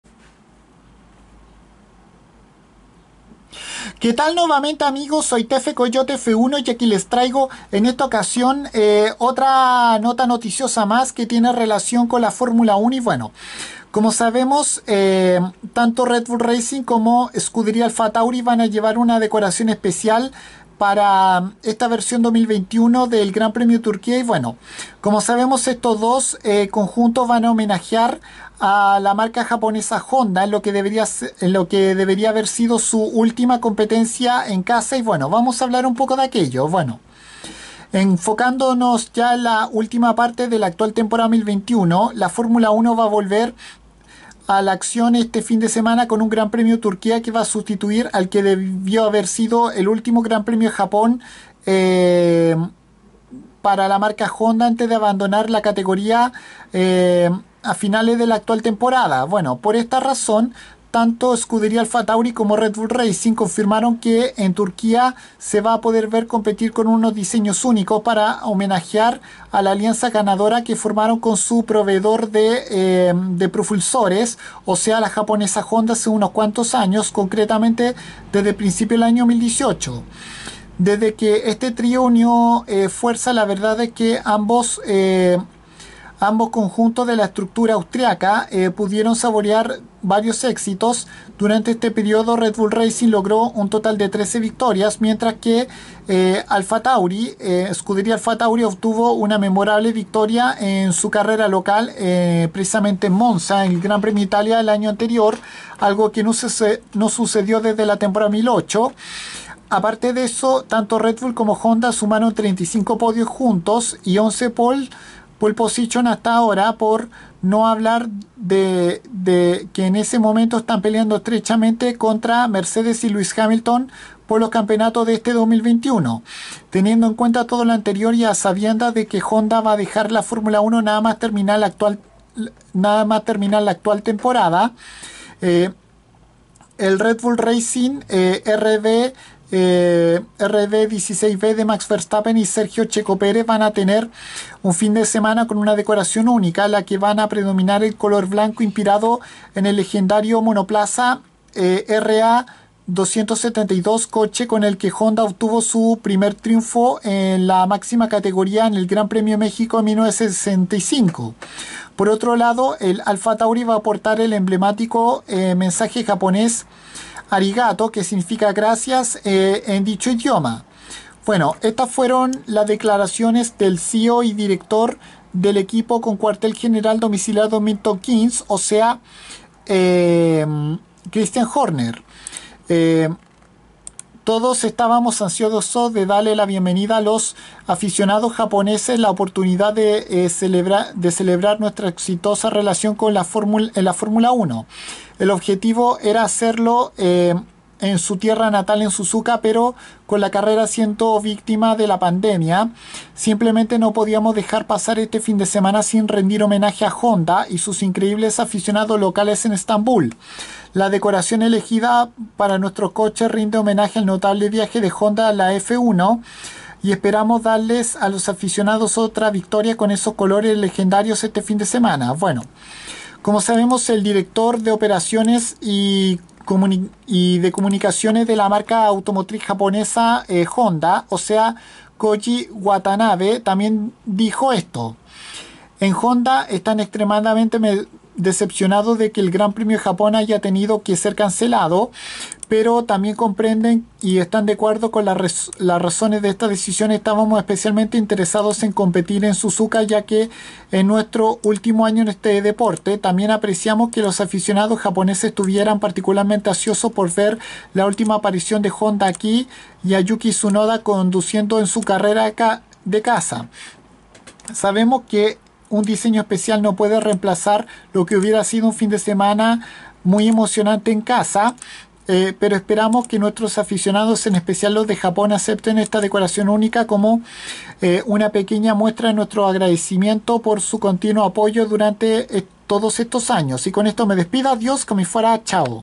Red Bull Racing y Scuderia Alpha Tauri tendrán decoraciones especiales para el GP de Turquía 2021
El motivo es por la última temporada de Honda en la Fórmula 1 en la que pudo haber sido su carrera de casa el Gran Premio de Japón que esta Temporada 2021 fue completamente cancelada a causa del Covid19